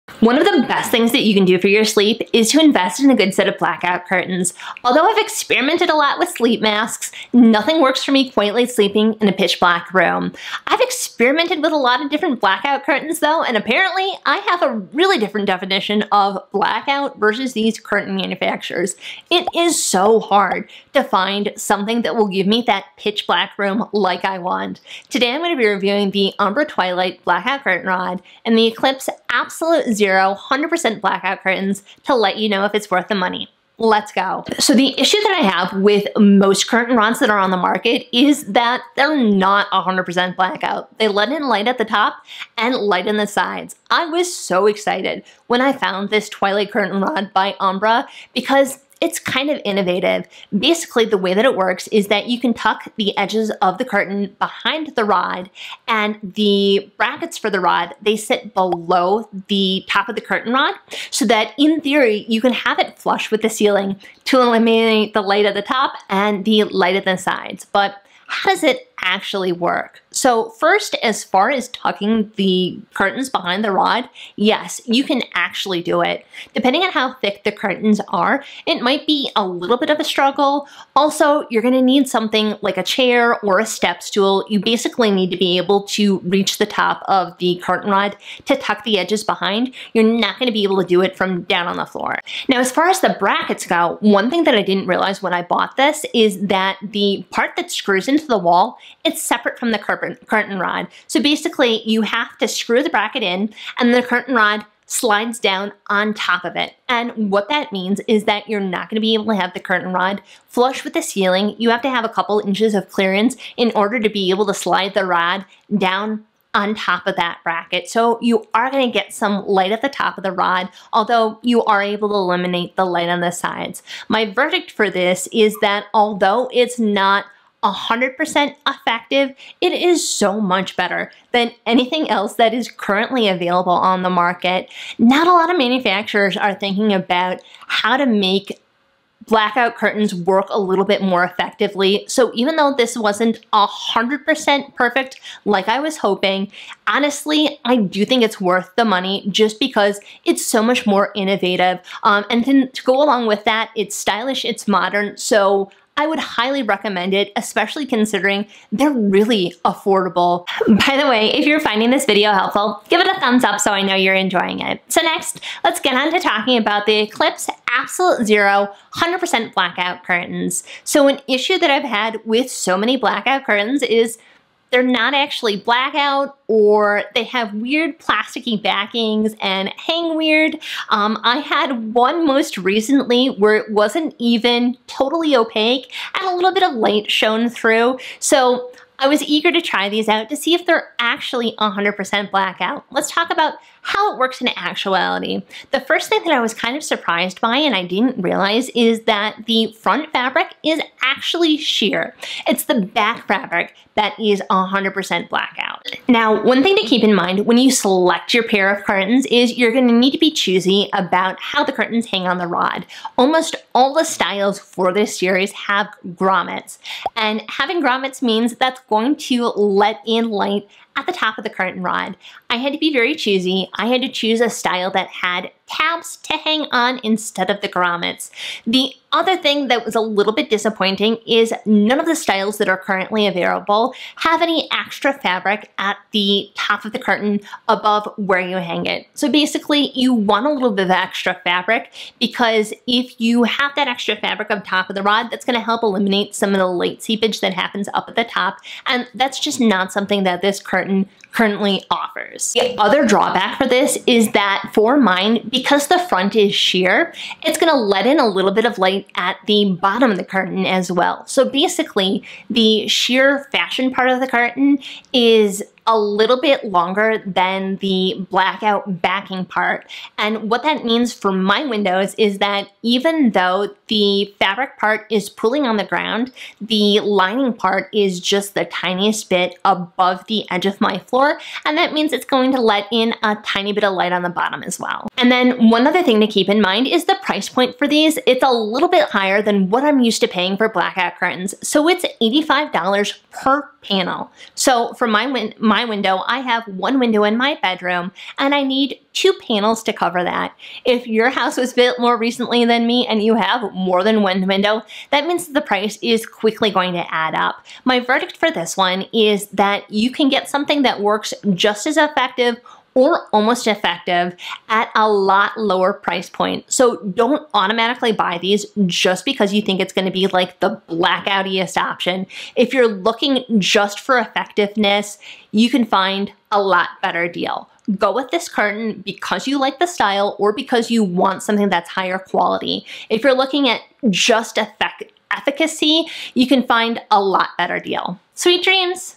Can I stop testing sleep masks!? Umbra Twilight Curtain Rod and Eclipse Absolute Zero Curtain Review
I’ve been searching for a better curtain rod and blackout curtains to give me a pitch black room.  Today I’m reviewing the Umbra Twilight Curtain Rod and the Eclipse Absolute Zero Curtains - will they give you 100% blackout to help you get better sleep?